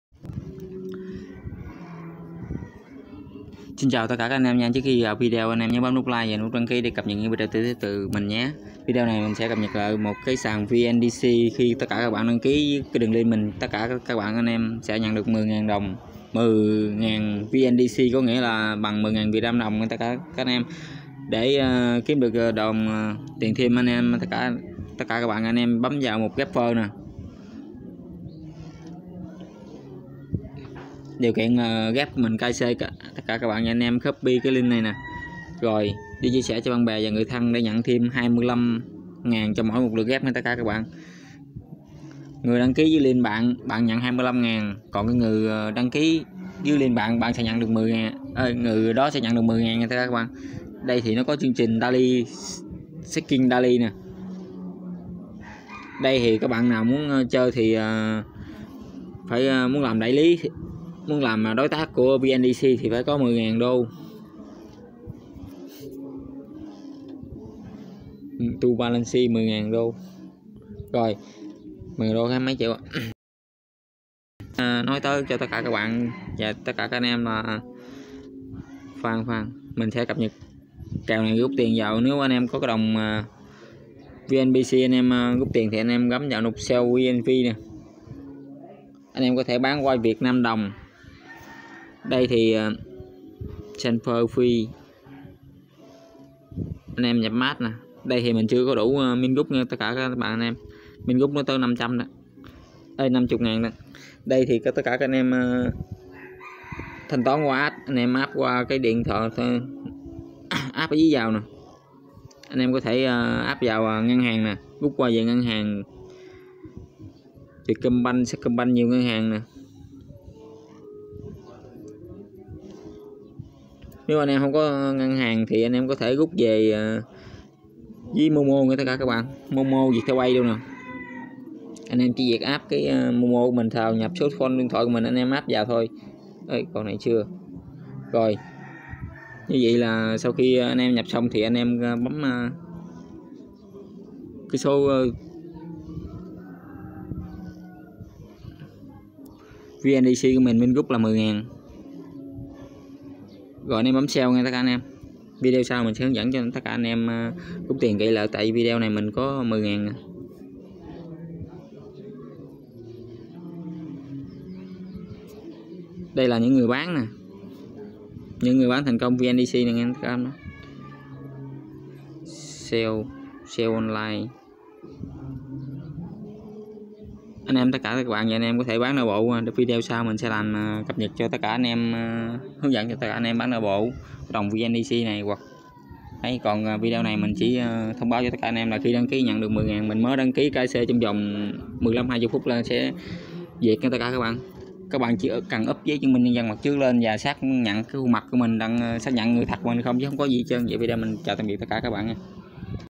kiếm 6trieu một tháng cùng VNDC_sàn giao dịch BTC Việt Nam
Link ĐK VNDC

video hướng dẫn đăng ký kiếm tiền từ VNDC